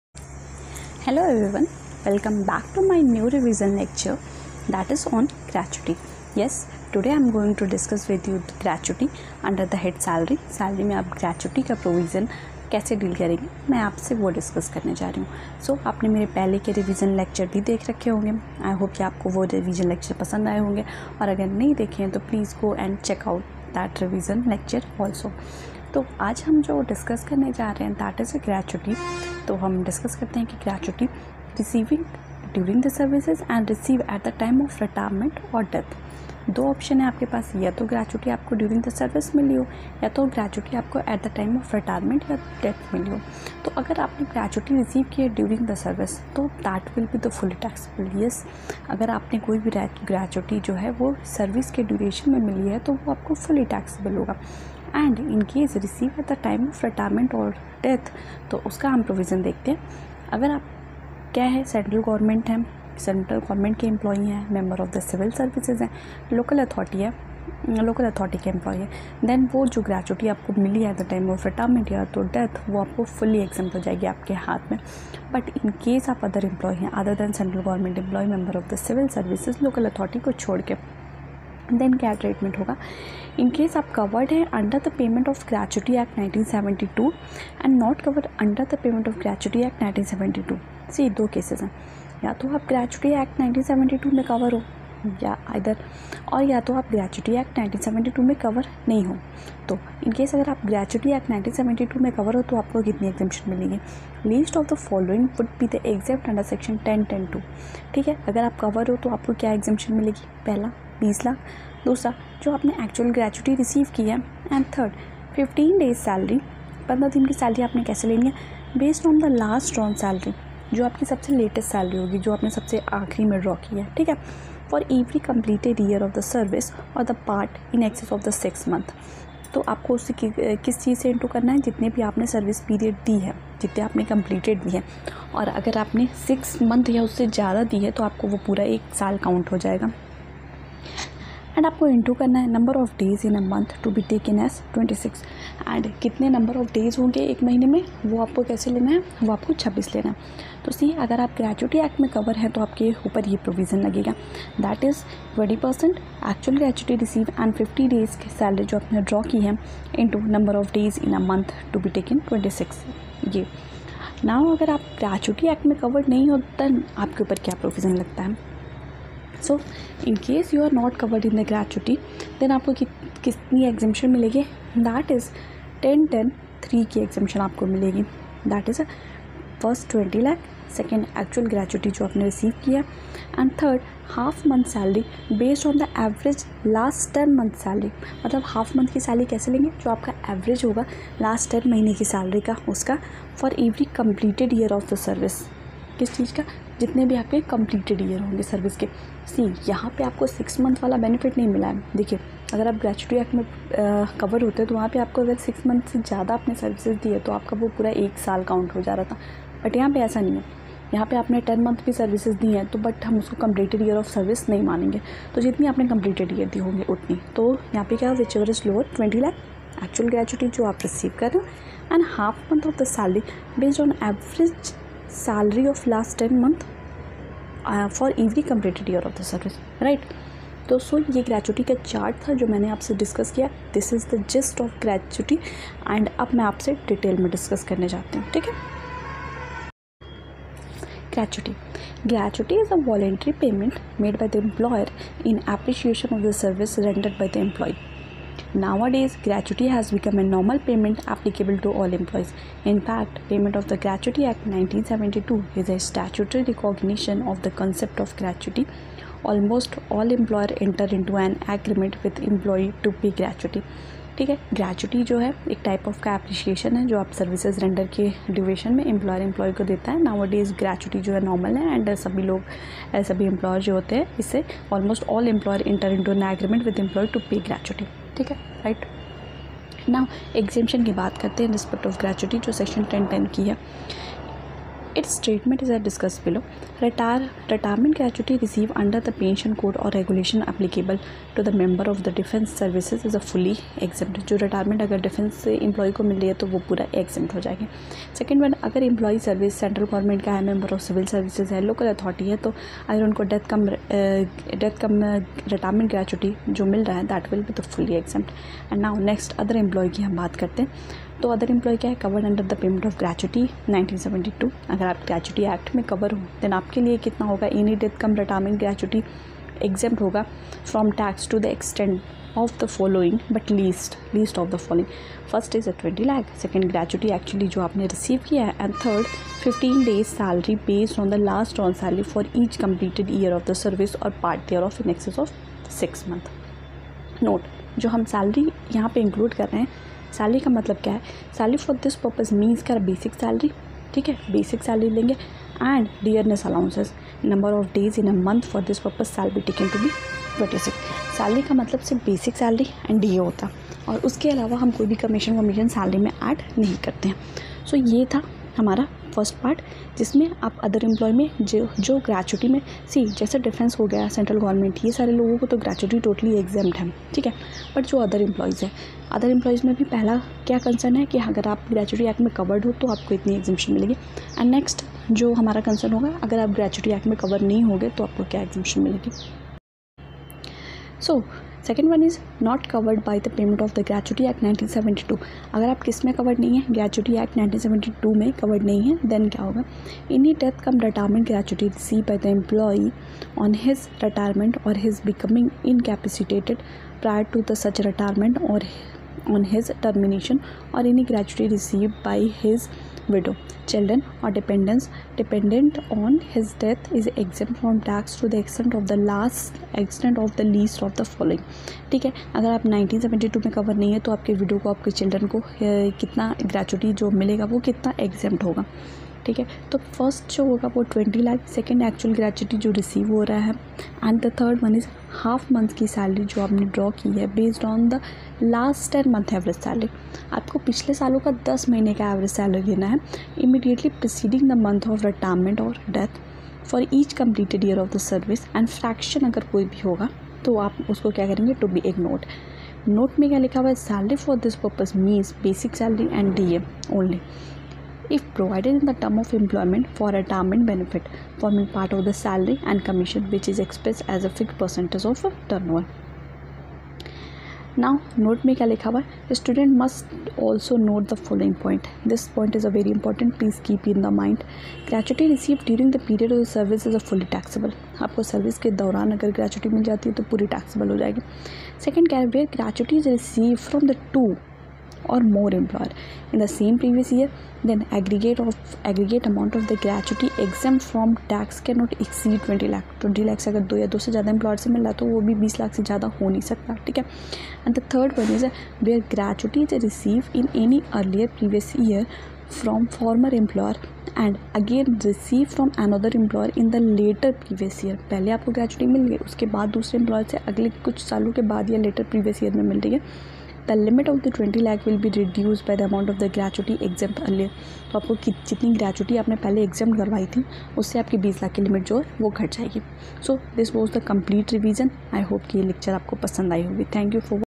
Gratuity Provision under Head Salary Full Revision
This video provide u complete gratuity Provision with Chart this will help you to understand Gratuity Provision easily.